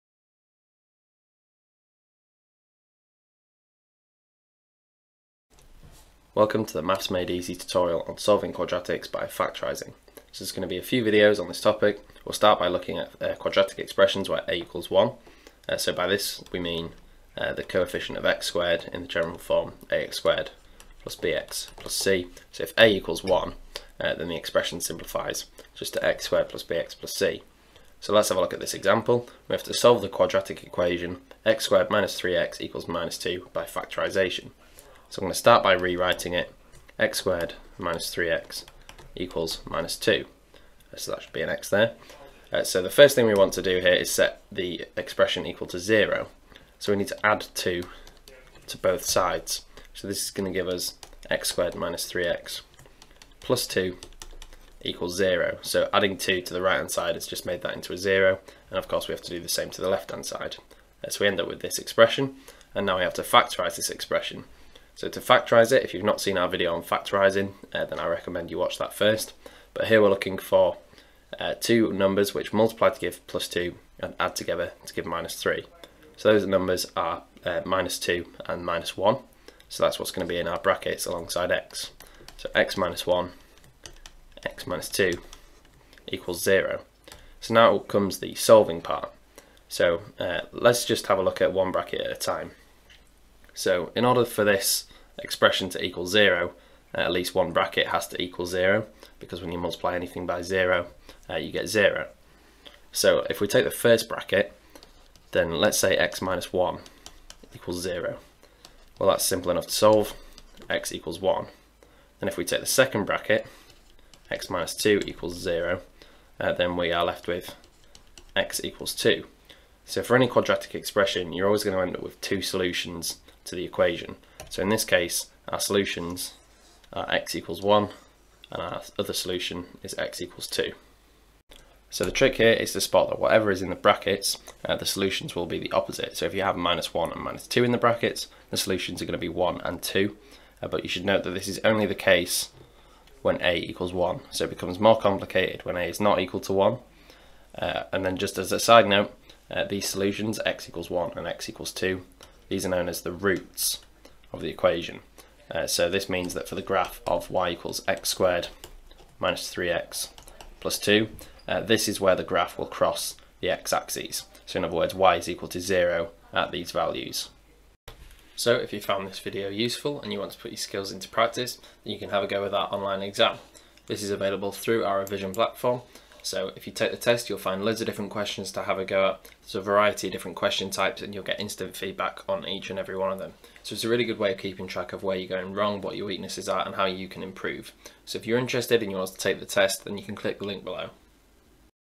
Algebra - Solving Quadratics by Factorising (a=1)
This is the Maths Made Easy video on Algebra - Solving Quadratics by Factorising (a=1)

To see our Algebra - Solving Quadratics by Factorising (a=1) online exam click the following

TIMESTAMPS
0:00 Intro
0:05 Skill 1: Solving by Factorising (a=1)
2:48 Outro